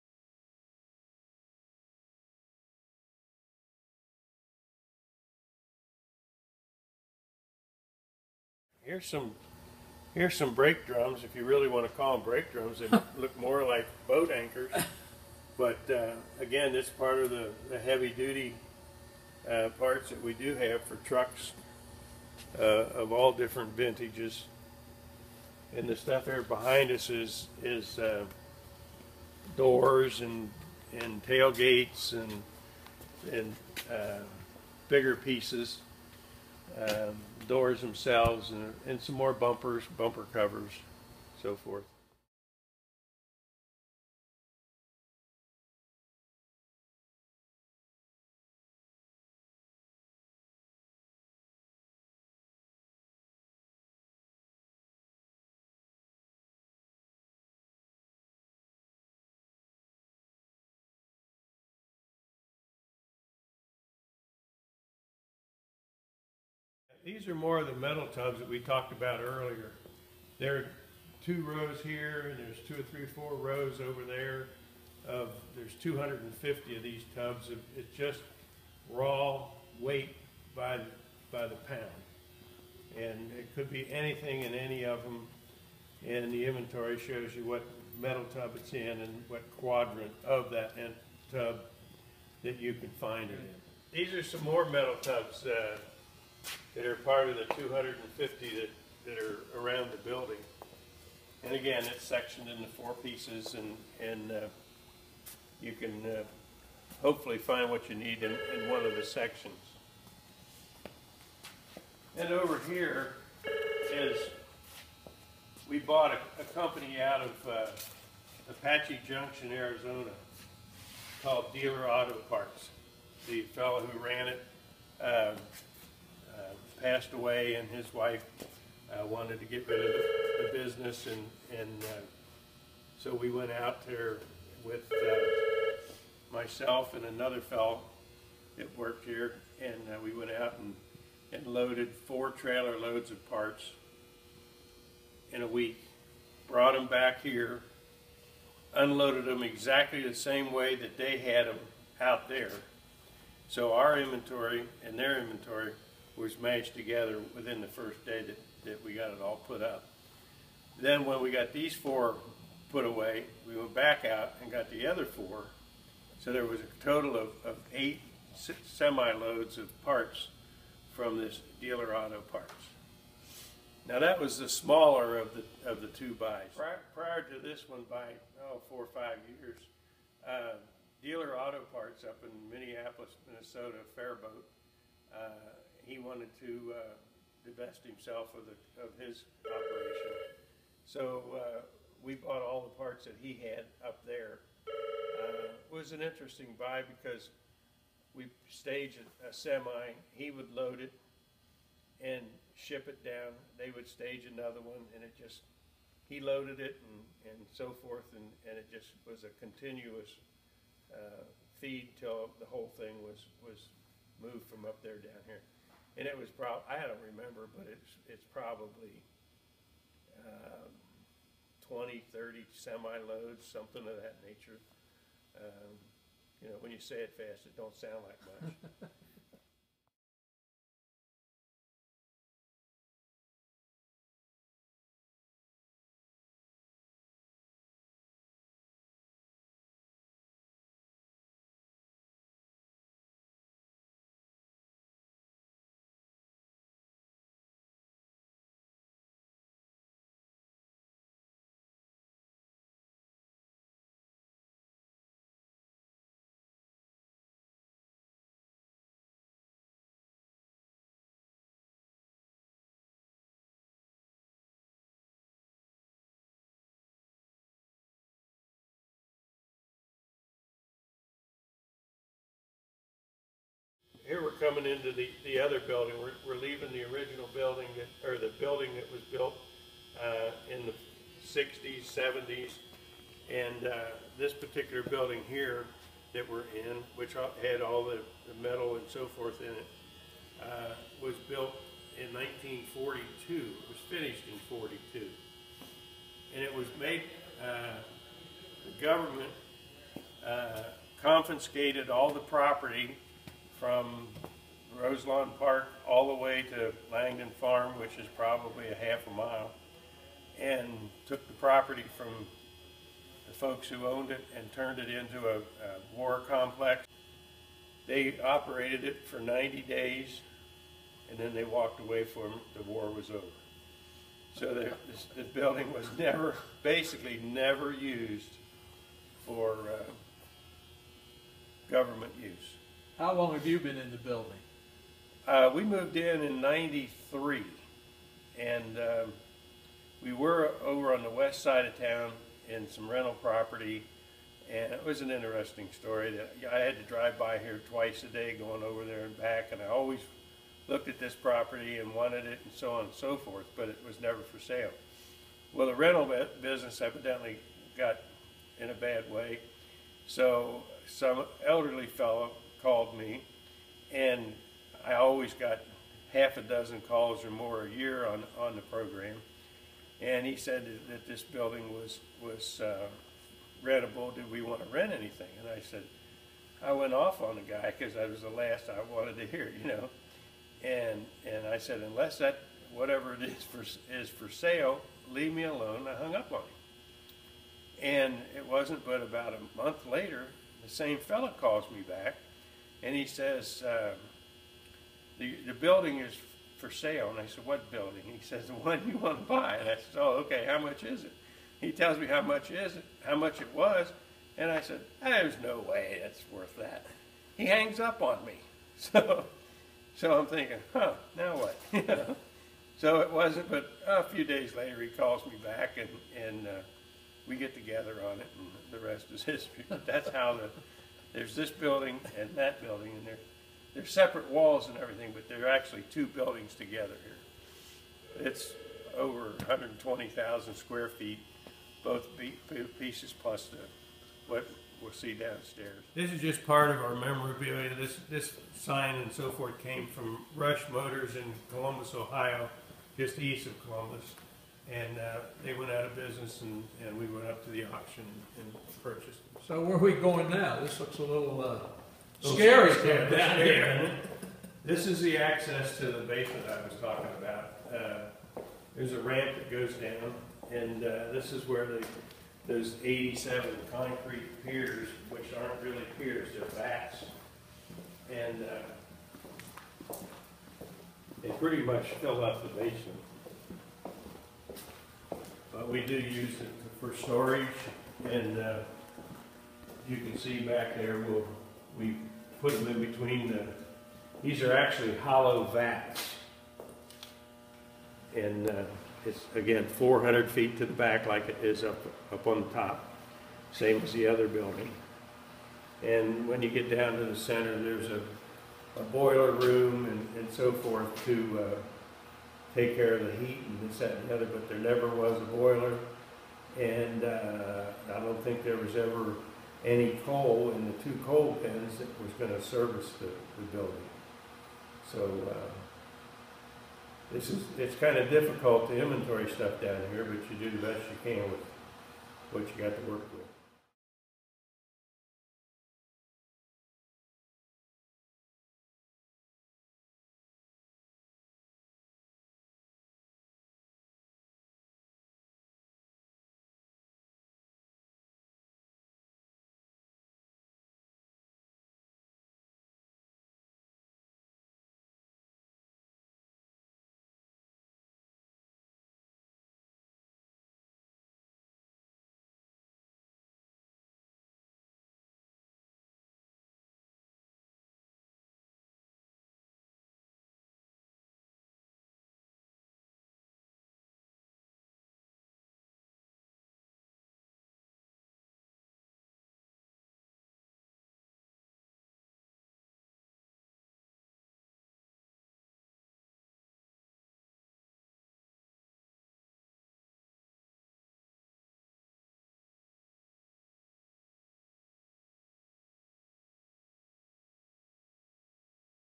Tour Green Sales Company Part 3
Tour the worlds largest obsolete ford parts warehouse.  Green Sales Company is an independent Ford and Lincoln-Mercury parts warehouse. Since 1957 we have bought from and sold to Ford and L-M dealers, service centers, hobbyist, and individuals around the world. Our inventory of 220,000 Ford/Lincoln-Mercury part numbers is 99% new original equipment covering 1950 to present.  We also have a wide selection of reproduction items. All Ford product lines are represented. Essentially, we may carry anything a dealer normally would have stocked over the past 51 years.

 We specialize in brakes parts; suspension; steering boxes; crash parts; weatherstrips; mouldings; gas tanks; water pumps; exhaust manifolds; switches; over 1500 fenders and quarters and items you just are not going to find elsewhere.  Take the Green Sales tour to view a few scenes of our 110,000 sf. facility.